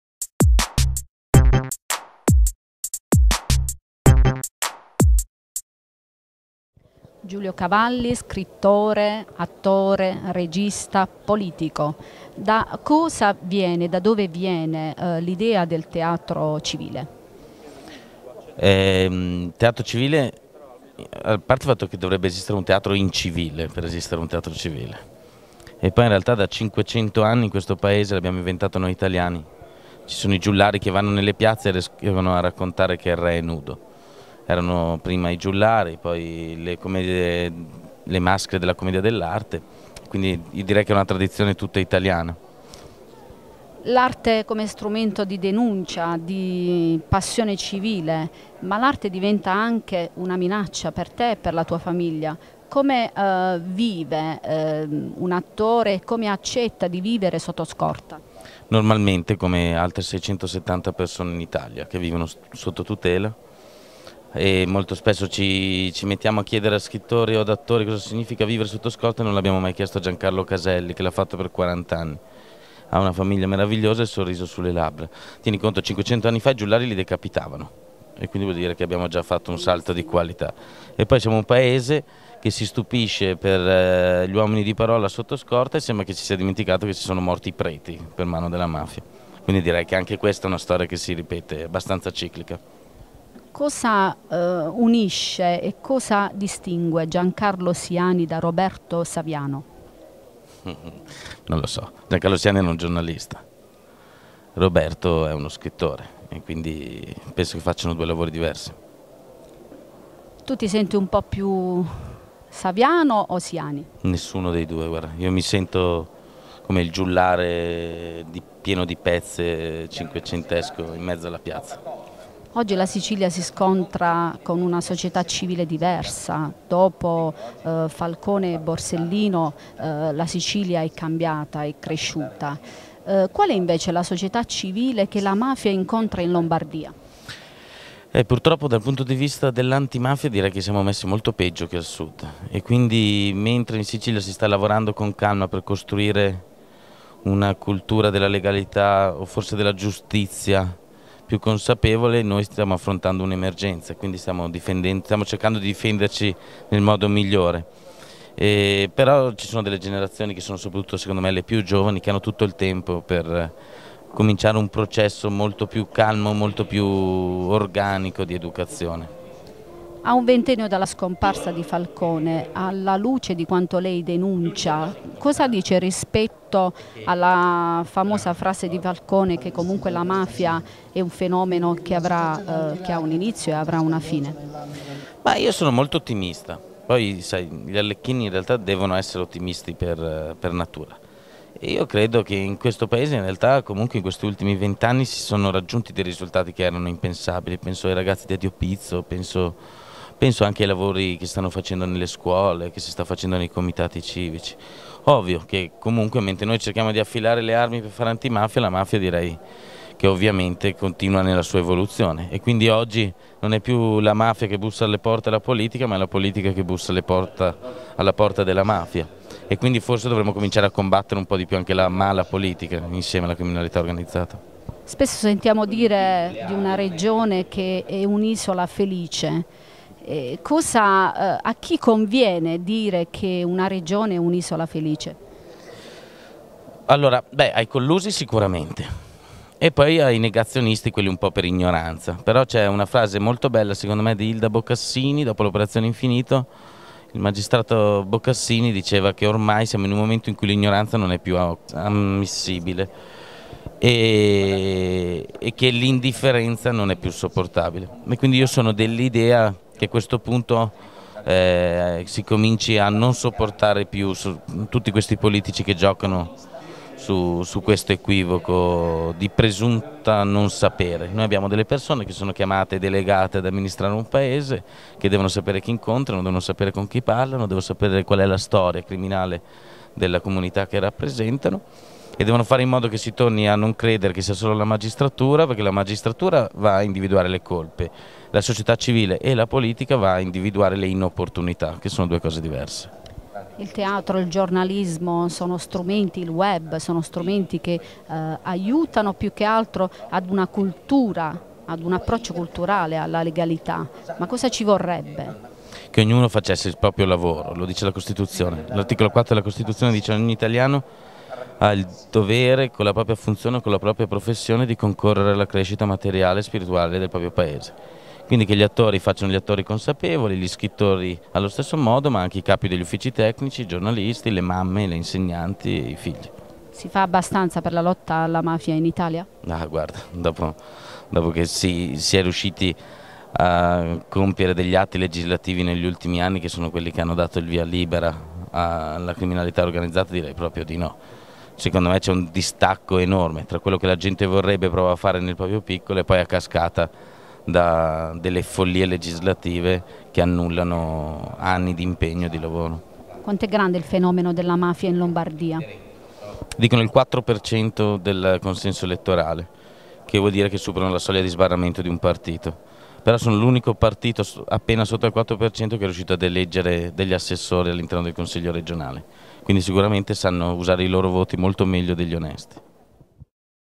Maria Andriulli intervista l'attore-scrittore Giulio Cavalli
Nell'ambito della ''Giornata Antimafia'' organizzata dall'Associazione Antiracket Falcone - Borsellino, tenutasi nell' Abbazia Benedettina di   Montescaglioso, il 30 luglio scorso,  Maria Andriulli della redazione giornalistica di SUDITALIAVIDEO ha intervistato l'attore-scrittore Giulio Cavalli